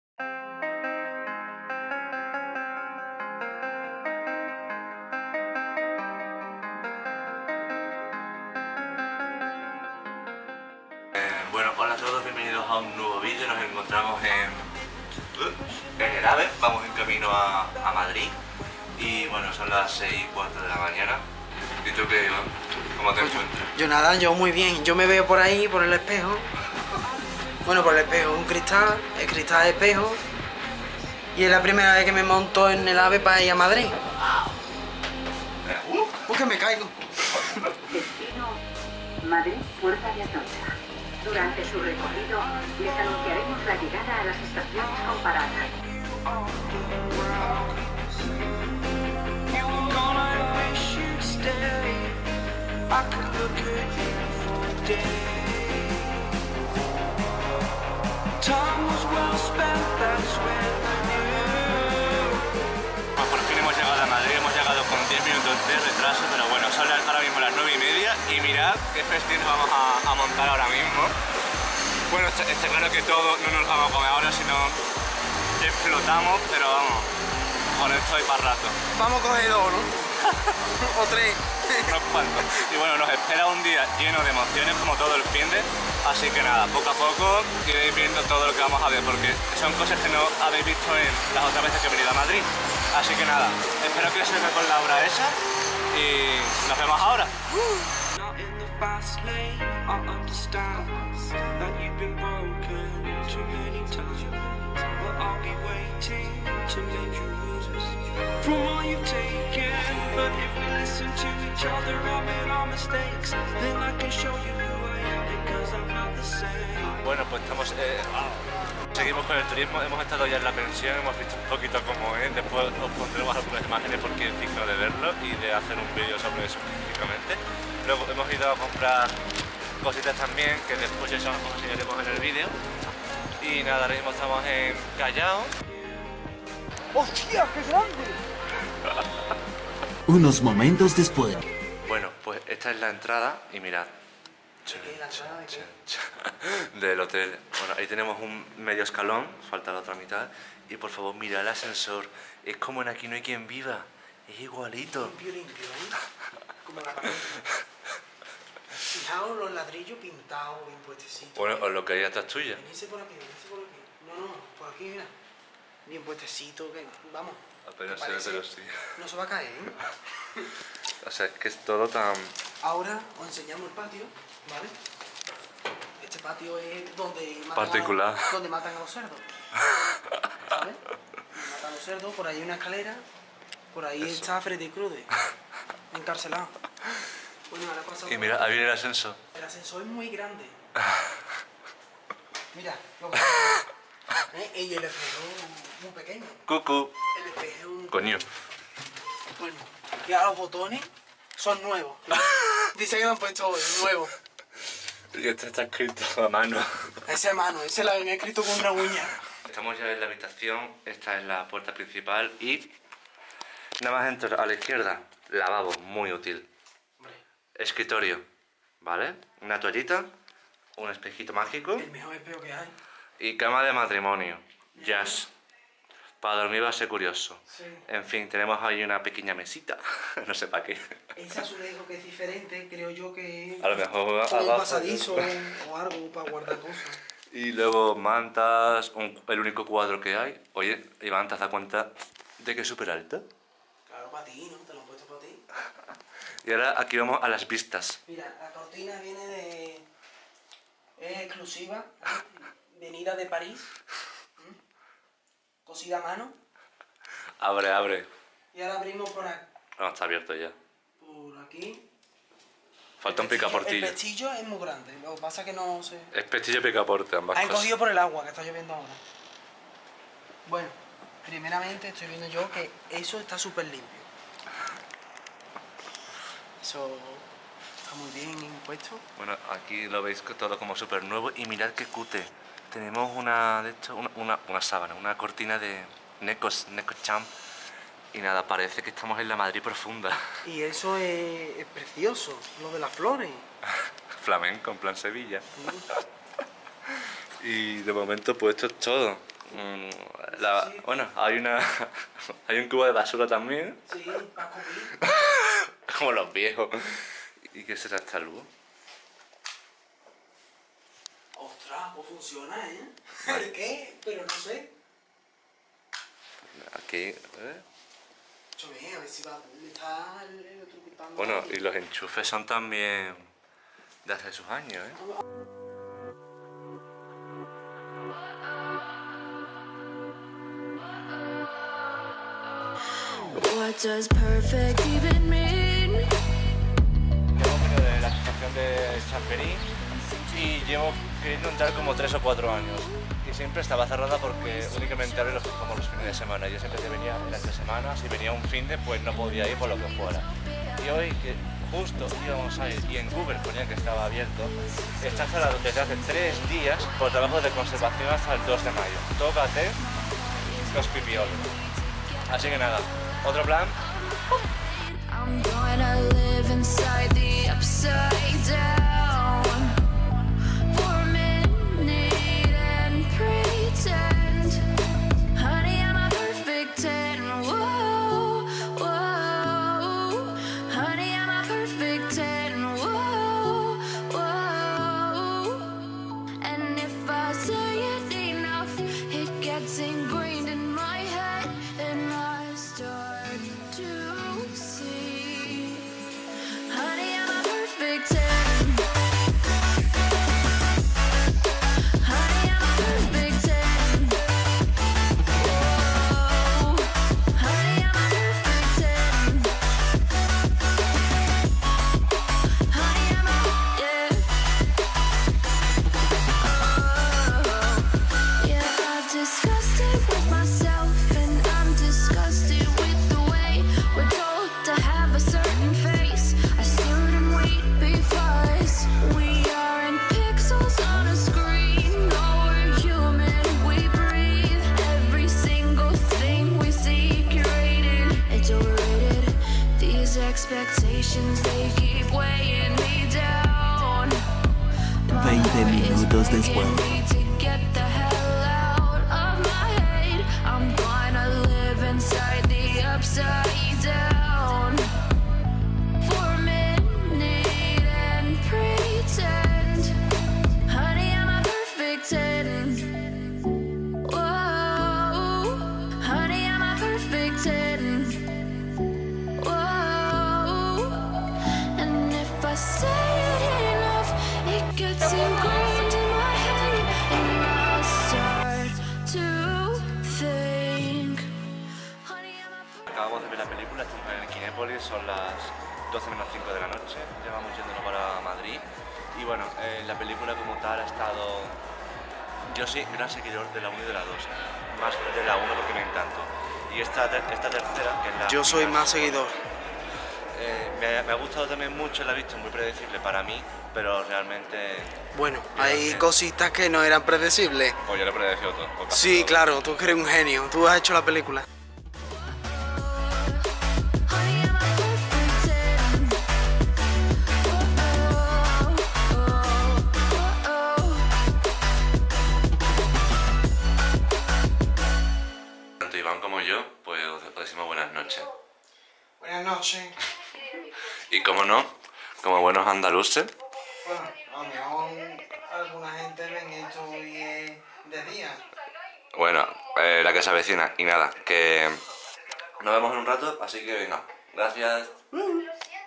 TurEx: La tarjeta 0 y el ascensor de ANHQV
Volvemos a Madrid por todo lo alto pro mi cumple para ver un espectáculo que vendrá en el próximo vídeo, para este mientras tanto un poco de turismo así como de descubrimiento por nuevos lares.

Hasta otra ;D
Un besote y a Cuidarse!